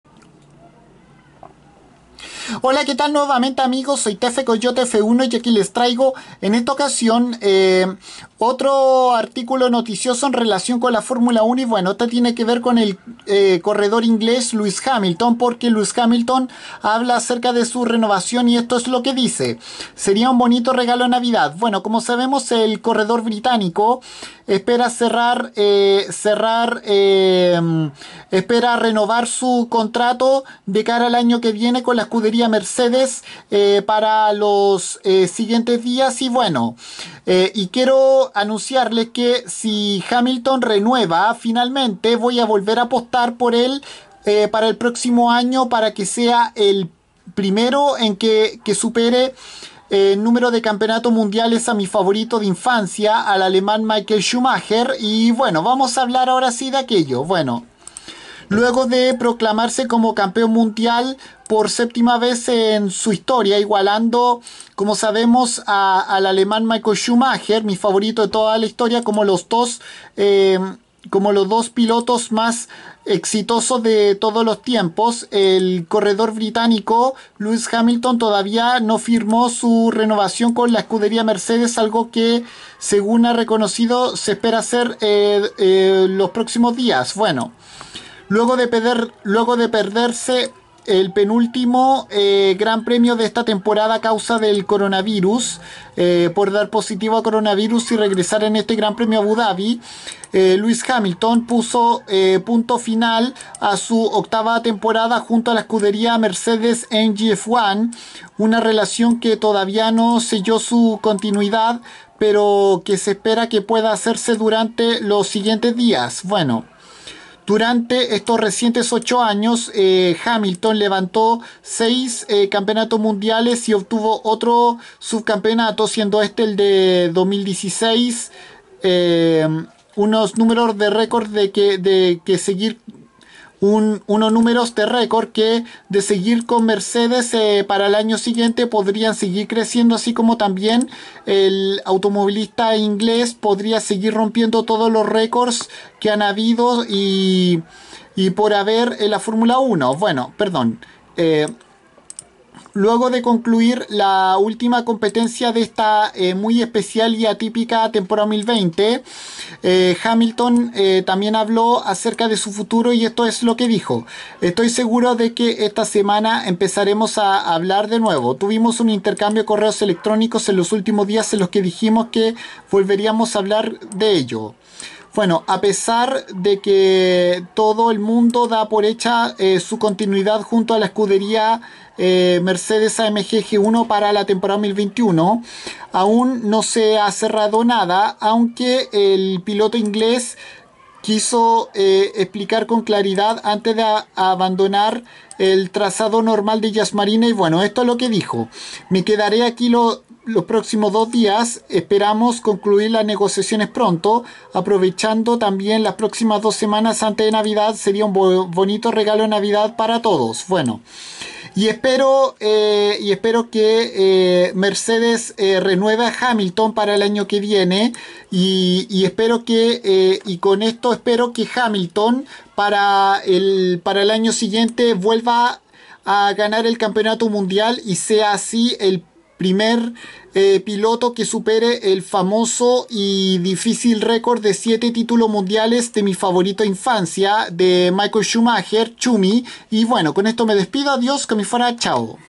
Lewis Hamilton habló sobre su renovación con Mercedes AMG Petronas Formula One Team
Si el corredor inglés renueva con la escudería con cuartel central domiciliado en Brackley volveré a apostar por el mismo para la siguiente Temporada 2021 de Fórmula 1 para así ganar su 8vo título mundial y ser el primero en superar a mi favorito de toda la historia el alemán Michael Schumacher "Schumi" o "Kaiser"